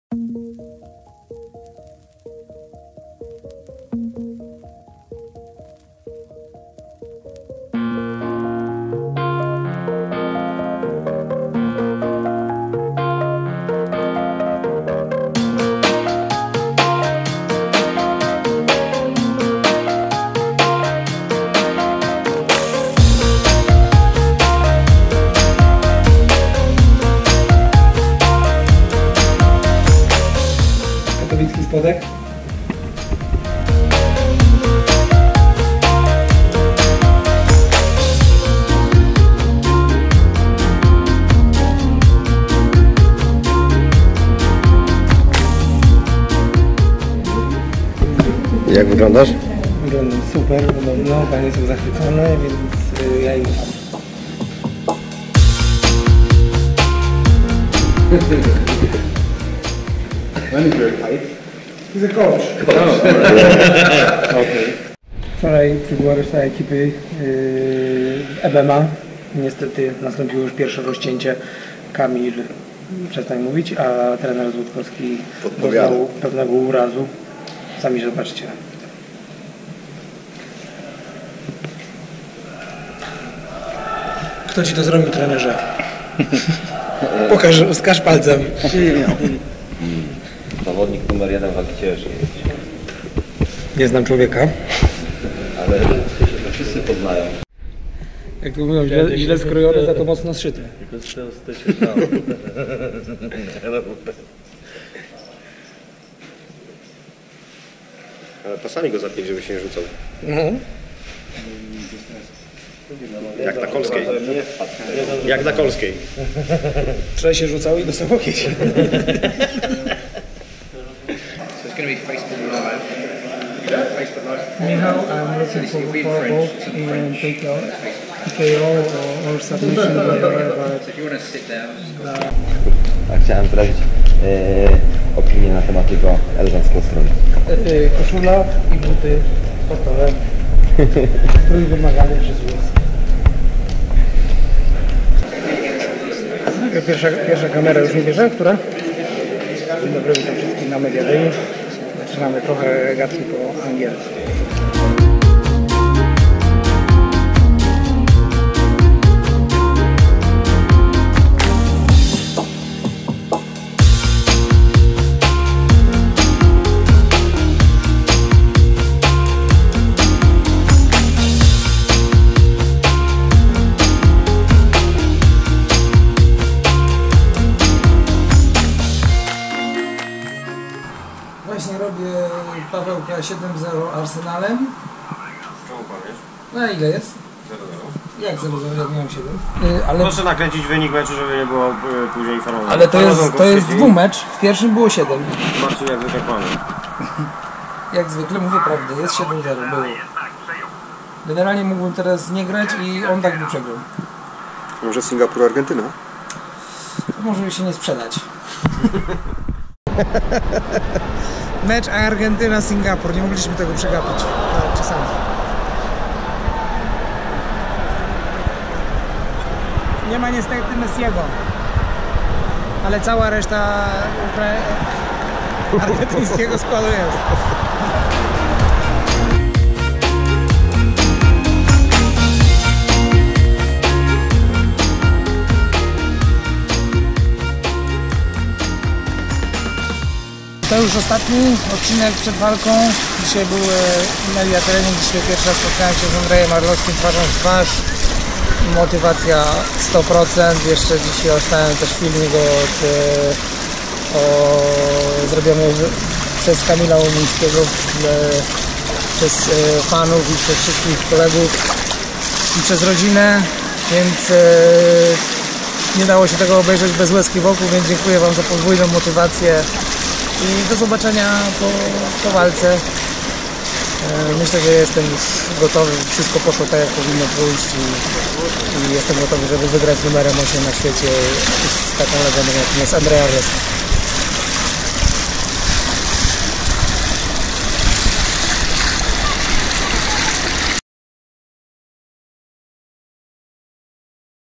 MARCIN TYBURA VLOG 5
Ostatni odcinek przed walką, która odbędzie się już 17 czerwca w Singapurze! Trzymamy kciuki za Marcina!!! :)